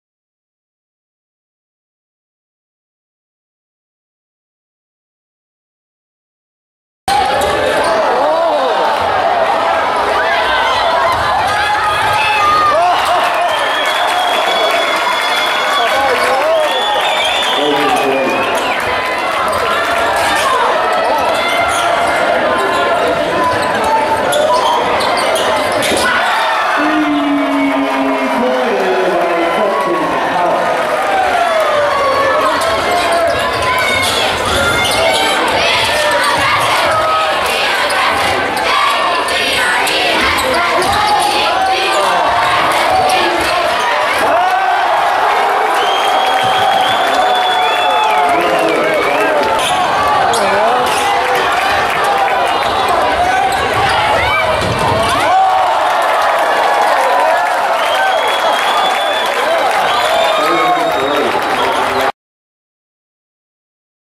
Santa Barbara Basketball
Highlights of Santa Barbara's win over Ventura.  The win clinched Santa Barbara high's 3rd straight league championship.